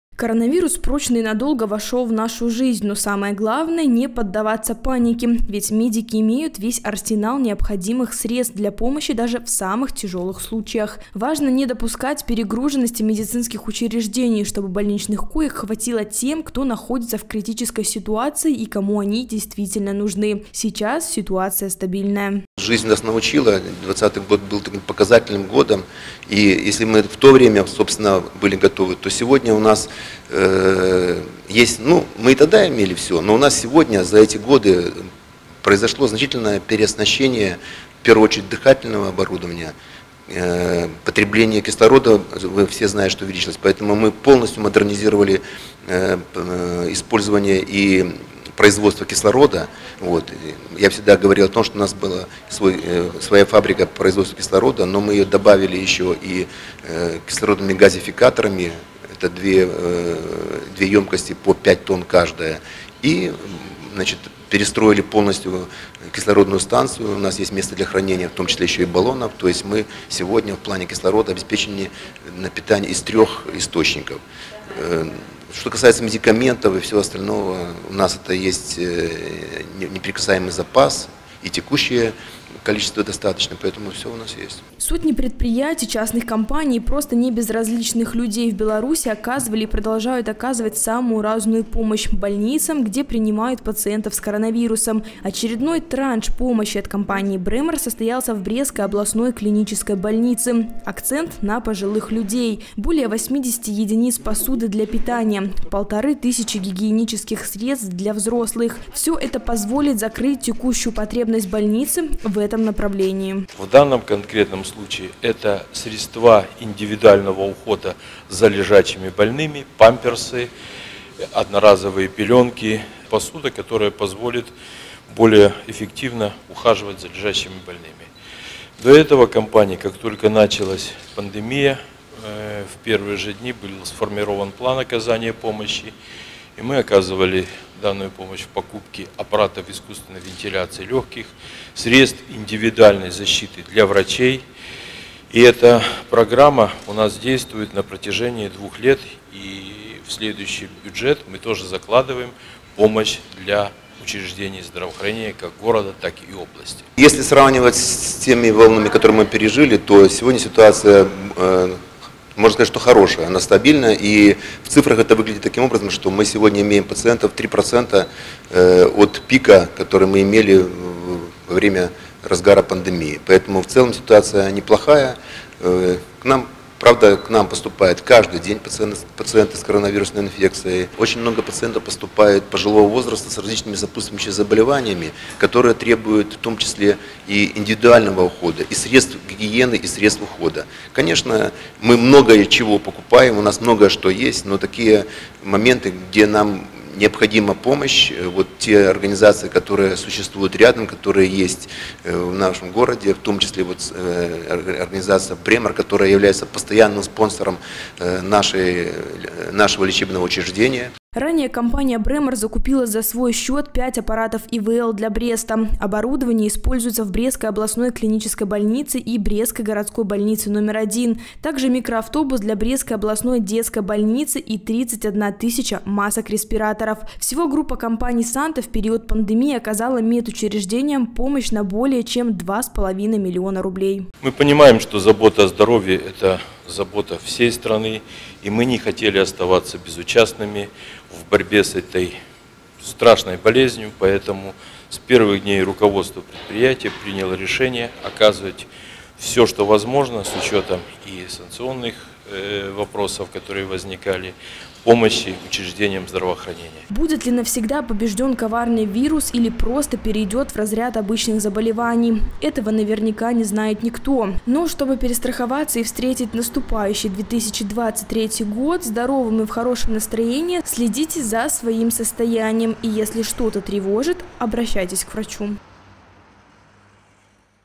2022-12-20 г. Брест. Передача средств гигиены компанией BREMOR. Новости на Буг-ТВ.
Передача средств гигиены компанией BREMOR Брестской областной клинической больнице.
Будет ли навсегда побежден коронавирус или просто перейдёт в разряд обычного ОРВИ – этого наверняка не знает никто. А потому тема ковида актуальна всегда. О ней говорят, пишут и показывают. Пример тому пресс-встреча с главврачом Брестской областной клинической больницы Александром Карпицким, где обсуждались вопросы по борьбе с COVID-19 и оказанию помощи медучреждению. Анастасия Мелишкевич продолжит тему